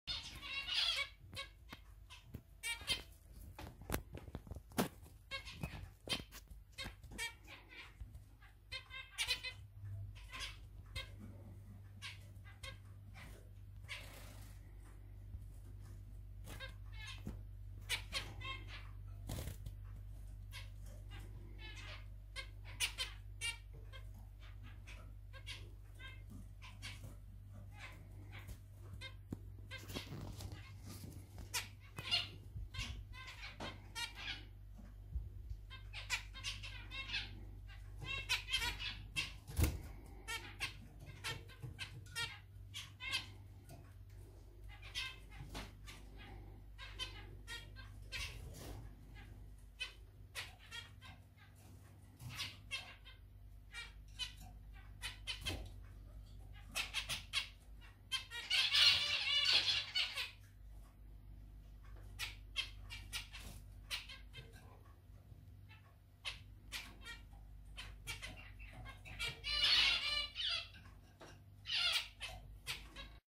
كم بيضة تبيض طيور الزيبرا 🐣 - المانيا
طيور الزيبرا فينش Zebra Finch - بيض الزيبرا والخصوبه
في هذا الفيديو اقدم طيور الذبيرا ولذي يبيض في كل مره 10او11 بيضه وهو الطائر نفسة اذا وضعته مع انثا يبيض وكذالك اذا وضعته مع الذكر بيصراحة لم افهم ما هو السبب

عصفور زيبرا هو أشهر أنواع العصافير ضمن فصيلة شمعية المنقار في وسط أستراليا. ينتشر عصفور زيبرا في معظم أنحاء القارة الأسترالية، ويتجنب العيش فقط في المناطق الباردة الرطبة الجنوبية وبعض المناطق الاستوائية في أقصى الشمال. يعيش عصفور زيبرا أيضا في موطنه الأصلي في جزيرة تيمور الإندونيسية، كما يتواجد في بورتوريكو